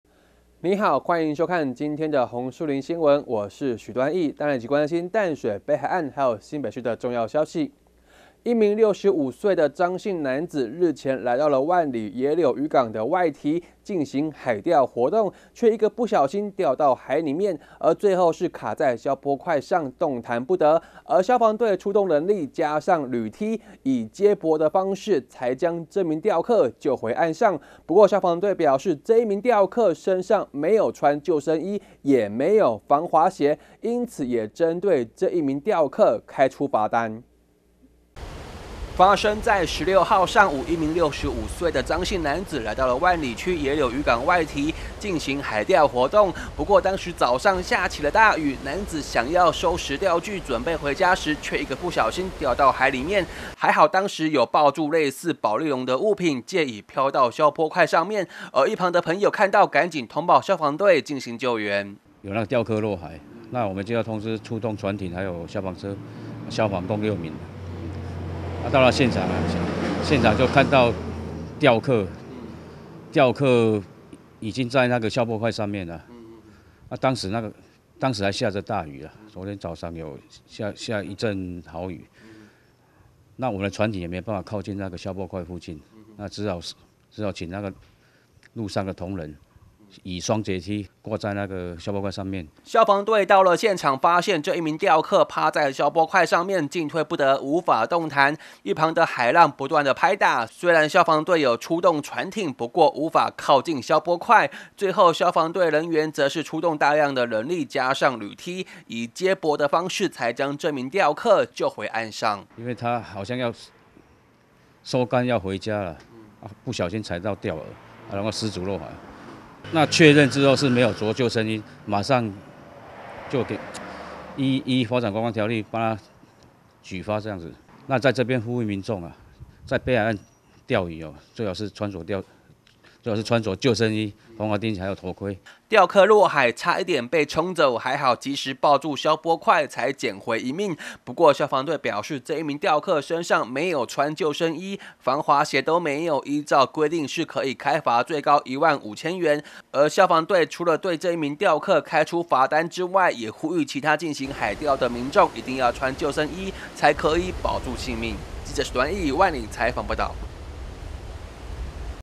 1040918紅樹林有線新聞 釣客落海卡消波塊救回 未穿救生衣挨罰
一名65歲的張姓男子，日前來到萬里區野柳漁港的外堤，進行海釣活動，卻一個不小心，掉到海中，最後是卡在消波塊上動彈不得。消防隊員則是出動人力，加上鋁梯，以接駁的方式才將這名釣客救回上岸。不過消防隊表示，這名釣客，身上沒有穿救生衣，防滑鞋都沒有，因此也針對這名釣客開出罰單。